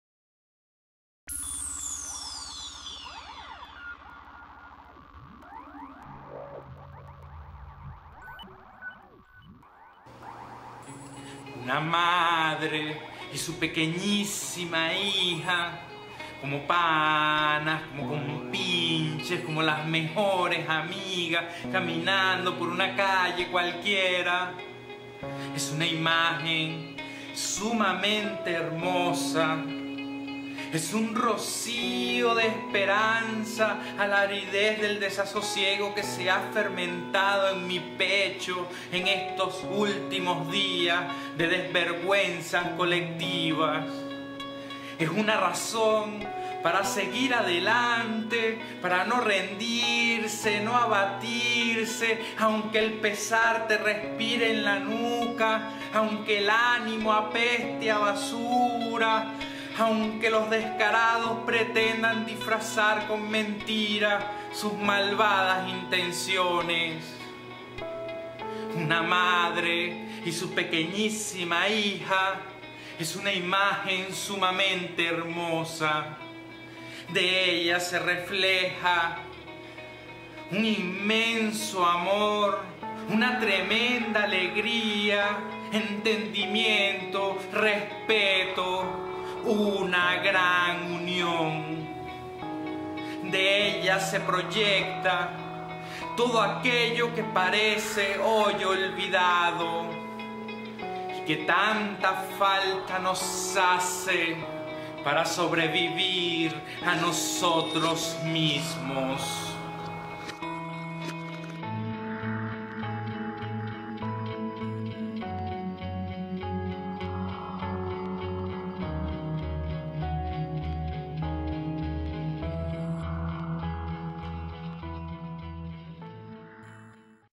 Poesía desde Valencia - APV2013: Aquiles González
Por el Día Mundial de la Poesía: Recital de Aquiles González, del Colectivo Poesía desde Valencia, para el Atentado Poético Venezuela 2013 (APV2013), enmarcado en el Encuentro Internacional de Poesía 2.0: "World Wide en Verso. Nuevos Medios. Mismas Palabras." de Ácracia pour les porcs.